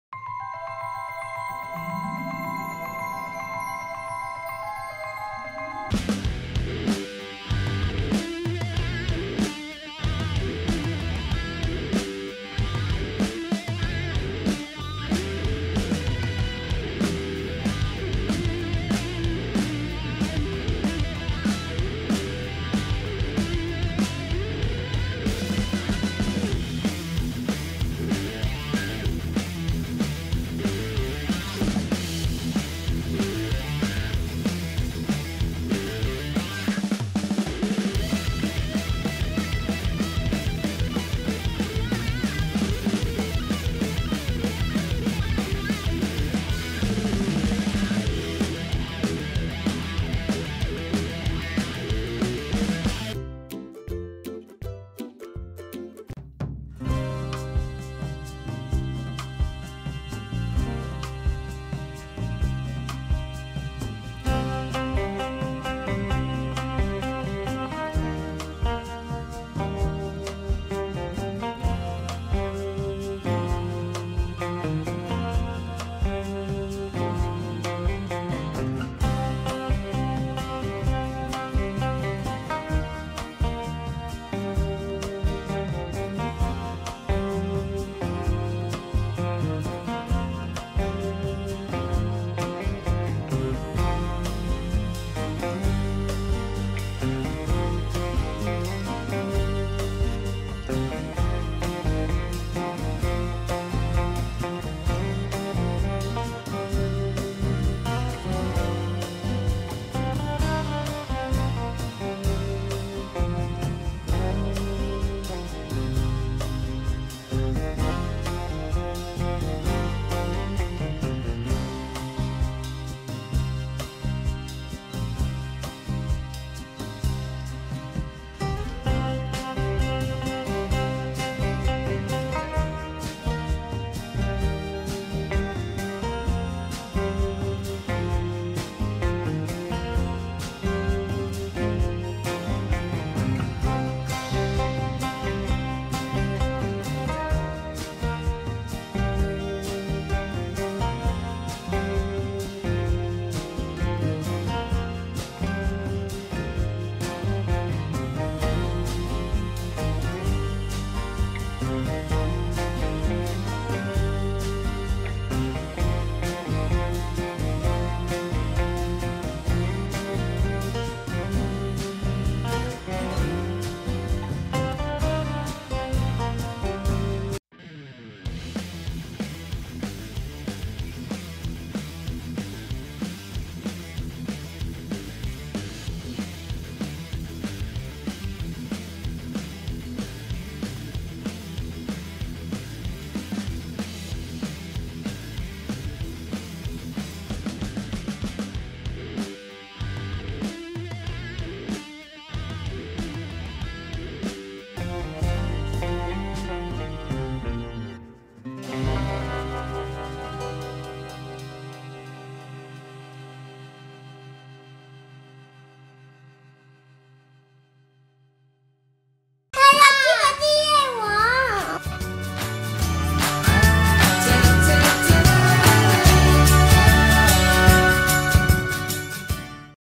大卷尾 VS 大冠鷲_EP08_(上)太過分了,大卷尾躲在樹上等大冠鷲飛出來_2023-06-06
2023年6月6日_窗外鳥世界_大冠鷲_大卷尾